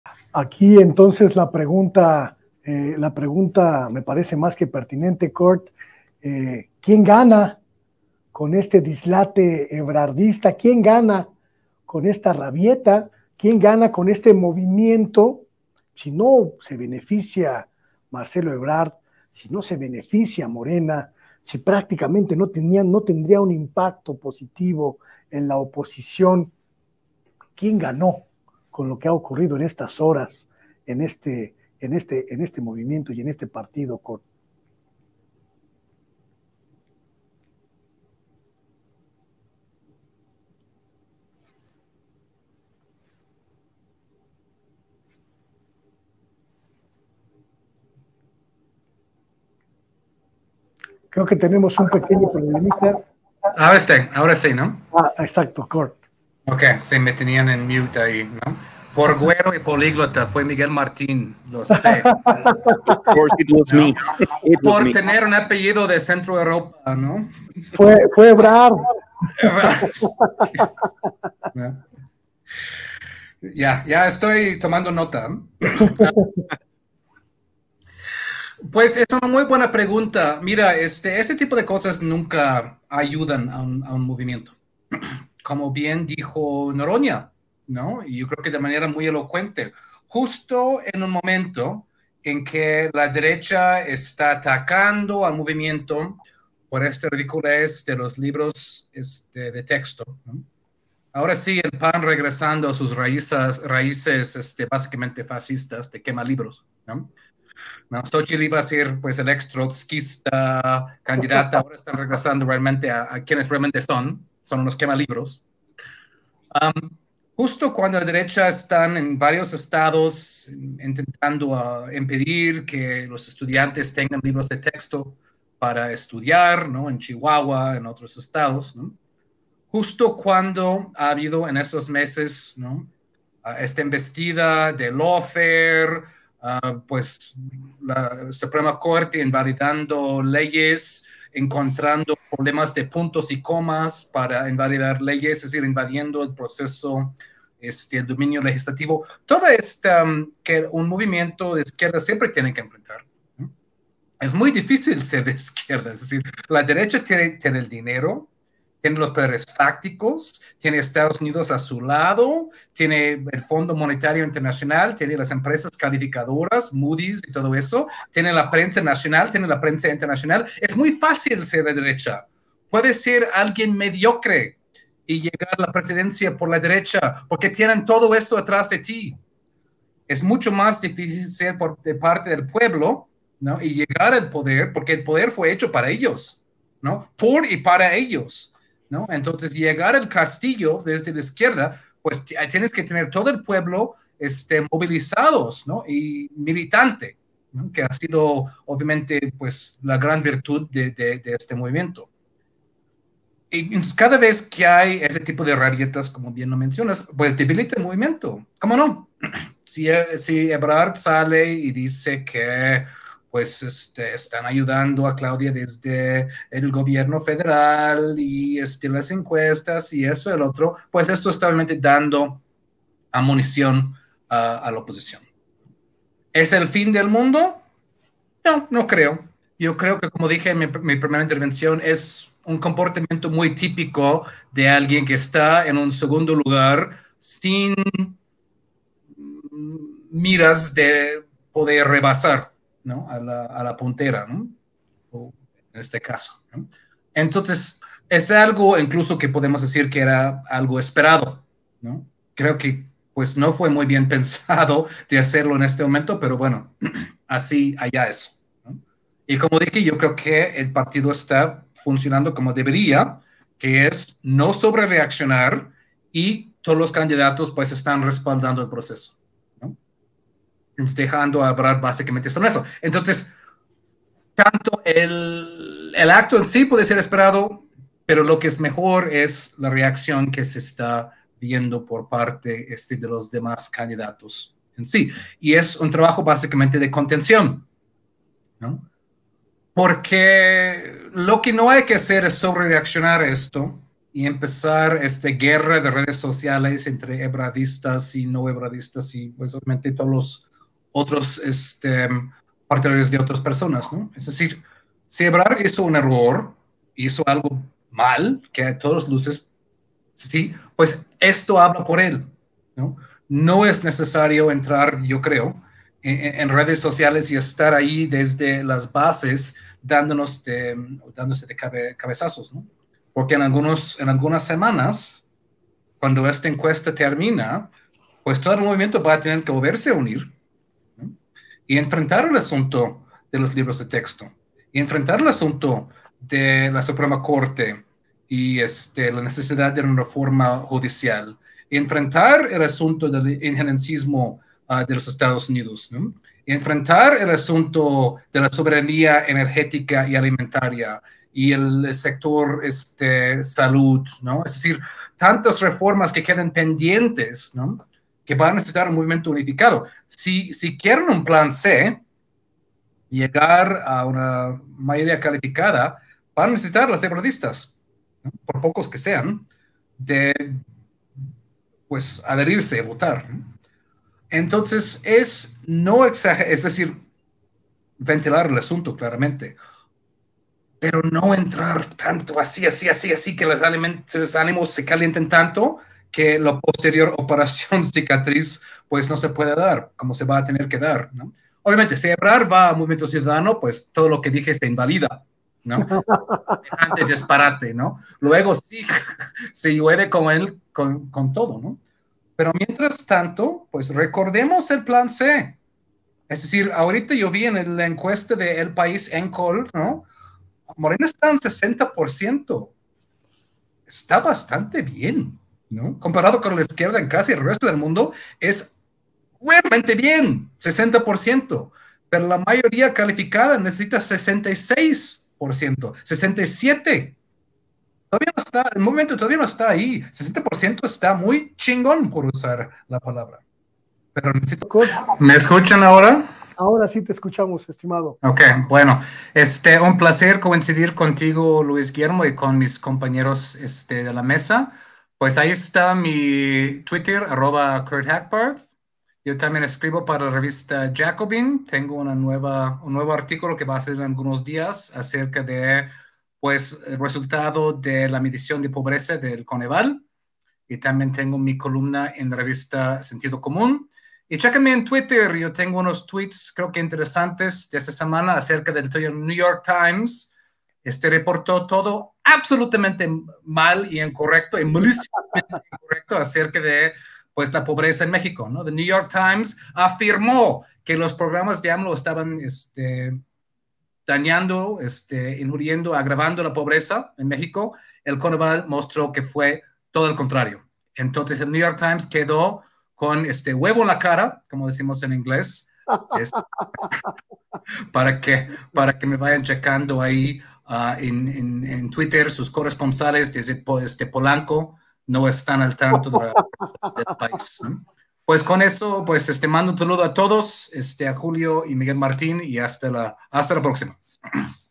K. HACKBARTH HABLA DE LA POSTURA DE EBRARD Y ASEGURA QUE ESAS RABIETAS DEBILITAN AL MOVIMIENTO
El escritor y periodista Kurt Hackbarth sostiene que en un momento en el que la oposición ataca tan insistentemente, ya sea con el tema de los libros de texto,o la Suprema Corte encontrando problemas en puntos y comas para invalidar leyes, el movimiento debe permanecer unido. Sin embargo, señala que el tipo de rabietas, cómo las que realizó el ex Secretario de Relaciones Exteriores, Marcelo Ebrard, debilita el movimiento, aún así, no es el fin del mundo.

Luis Guillermo Hernández: Periodista Independiente

Kurt Hackbarth: Escritor y periodista

Miguel Martín Felipe Valencia: Periodista y lingüista